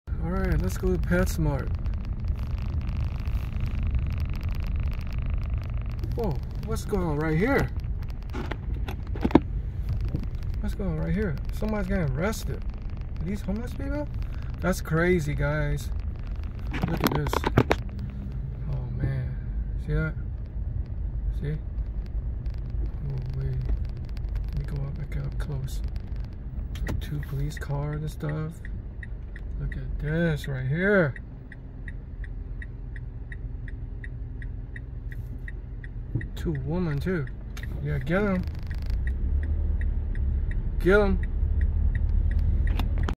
Bum VS 2 Female Cops
I don't know the story behind this but it looks like 2 female police officers are arresting this man while his girlfriend was distressed in the background.